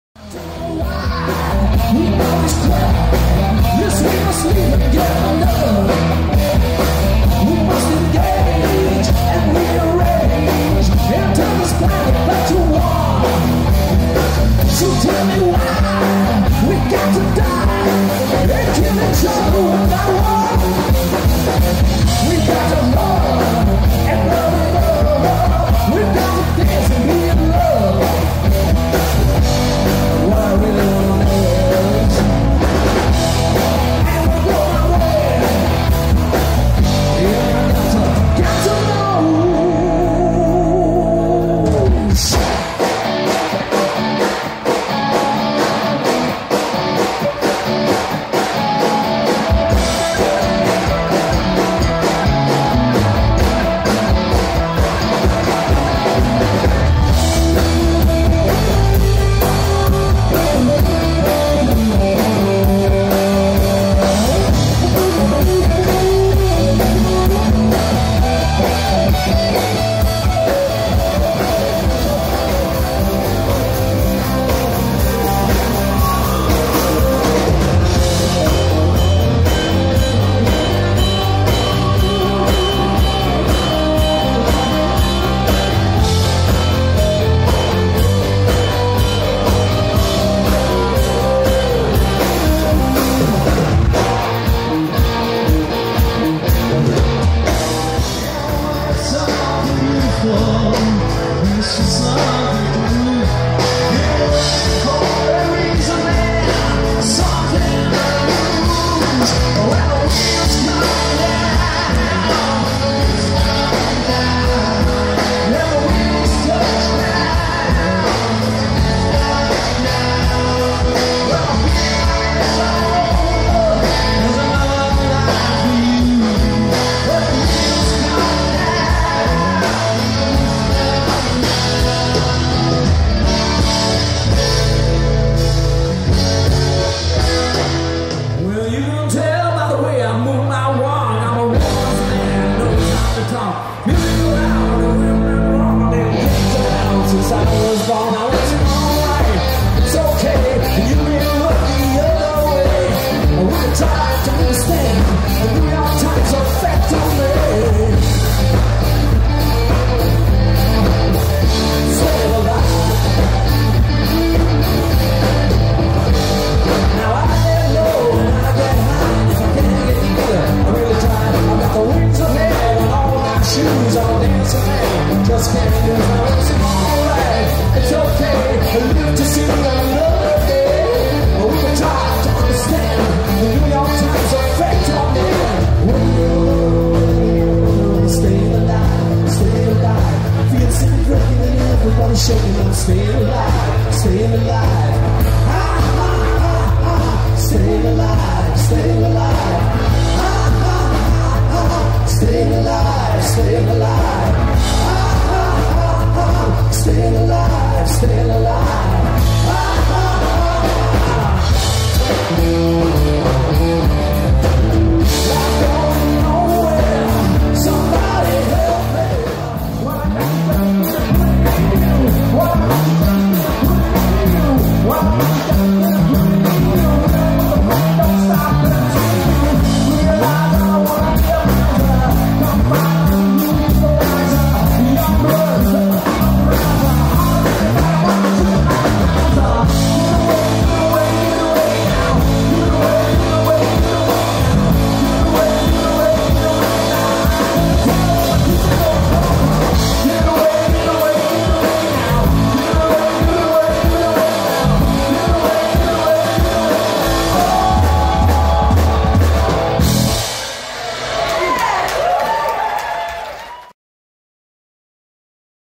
JELLYFACE live Heist-op-den-Berg 28 april 2018
Dit compilatiefilmpje werd opgenomen met smartphone beeld- en klankmateriaal en is aldus niet bewerkt. What you see is what you get.